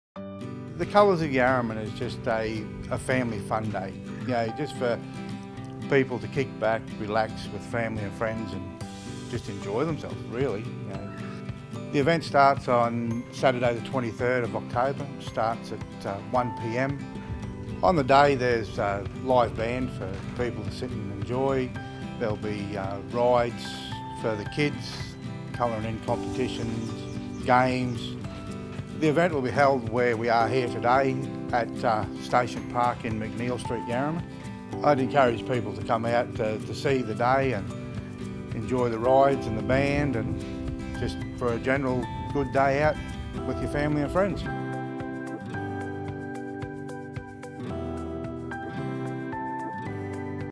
Colours of Yarraman Spring Festival 2021
🌷🎶 Join in the fun at the Colours of Yarraman Spring Festival this Saturday 23 October, 1pm to 5pm.

Come dressed in theme for the Mad Hatter's Tea Party and enjoy live music, free children's rides, food vans and more.